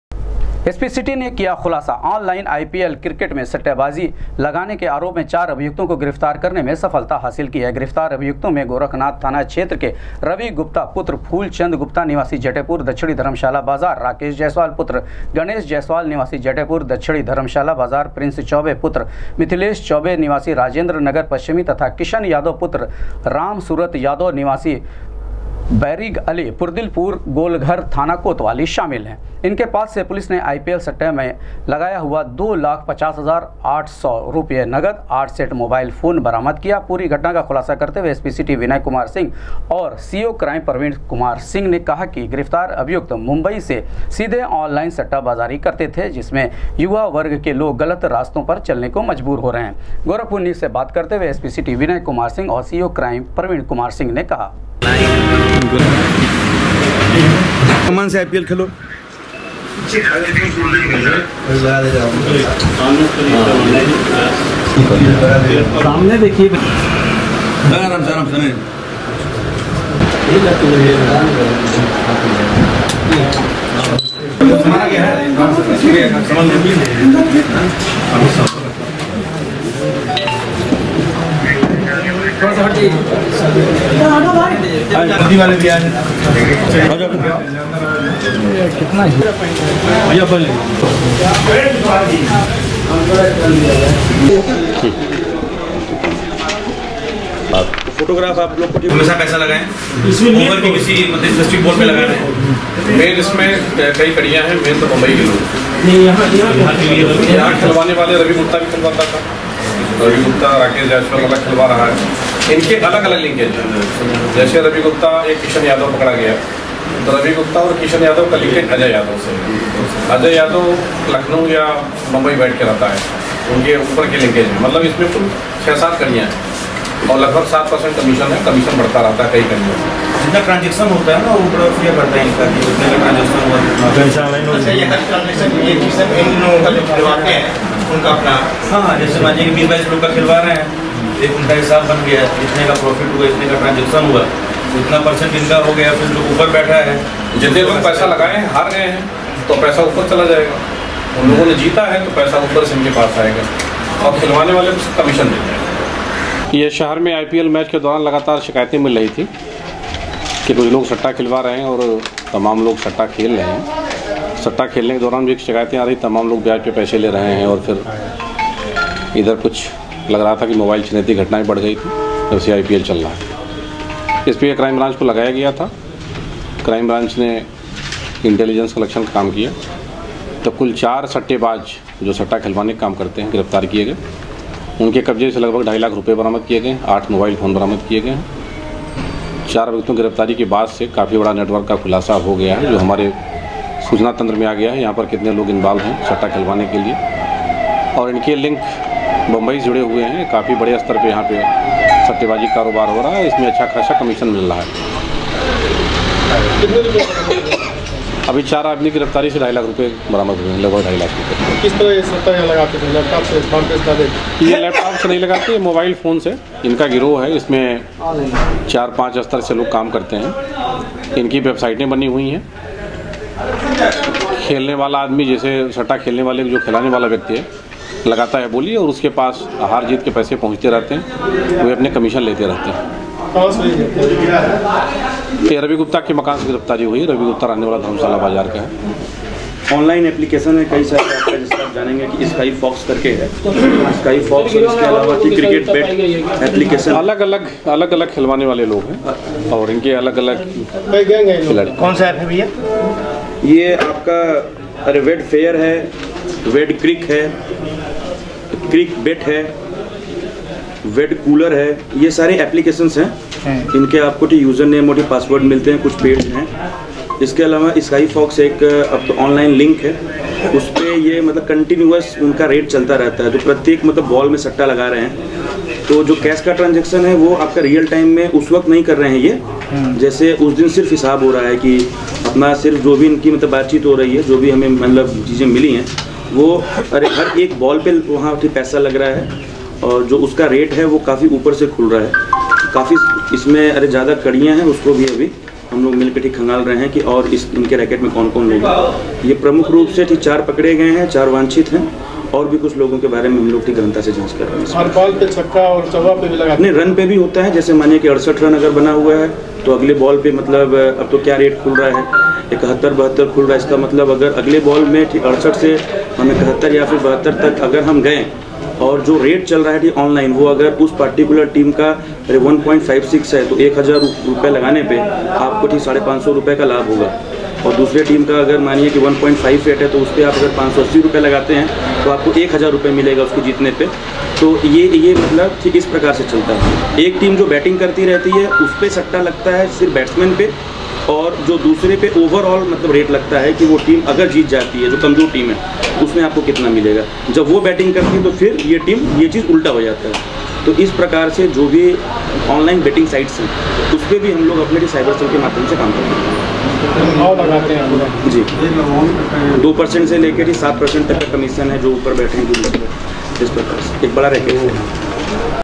आईपीएल क्रिकेट में आॅनलाइन सट्टाबाजी के आरोप में गोरखनाथ पुलिस और क्राइम ब्रांच की टीम
आईपीएल क्रिकेट में आॅनलाइन सट्टाबाजी के आरोप में  गोरखनाथ पुलिस और क्राइम ब्रांच की टीम ने चार सटोरियों को किया गिरफ्तार, दो लाख 50 हजार आठ सौ रूपया और आठ सेट मोबाइल भी किया बरामद